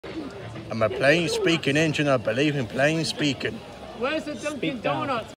Awdry Extravaganza 3 - Duncan is a plain speaking engine
Duncan believes in plain speaking